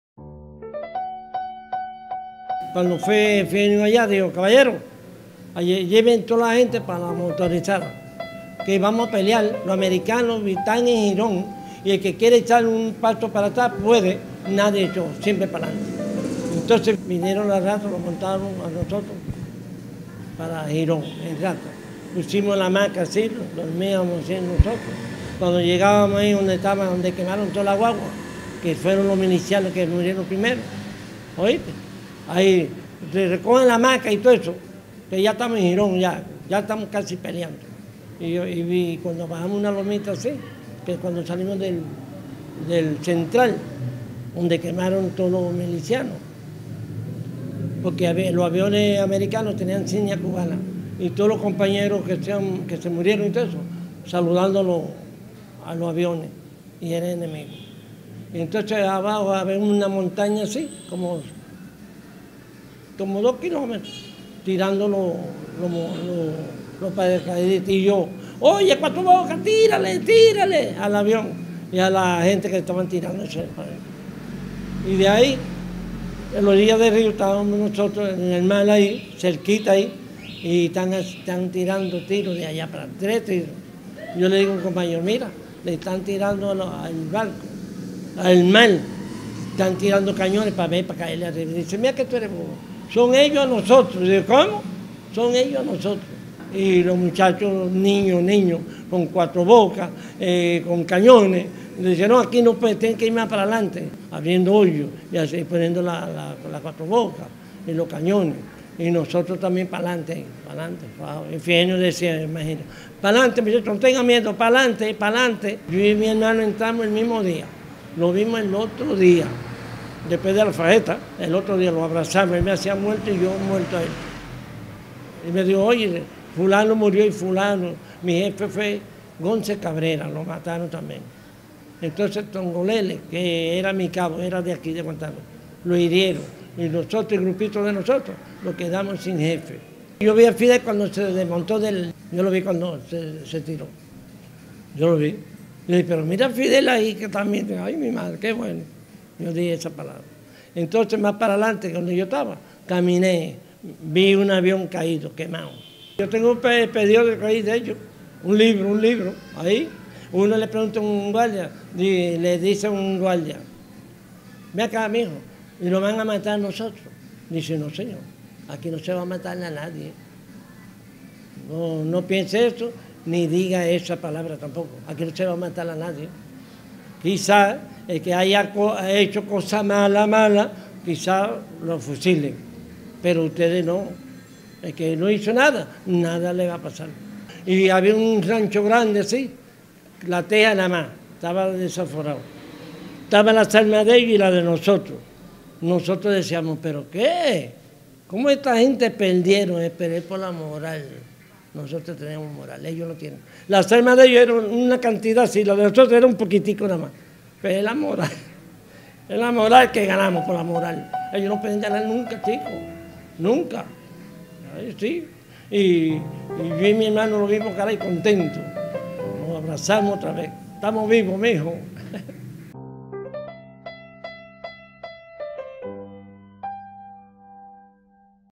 Héctor: un protagonista de Girón
Héctor recuerda claramente los gloriosos días de abril de 1961, cuando Cuba marcó la primera gran victoria del imperialismo en América... El periodista Isaac García Cárdenas nos acerca a sutestimonio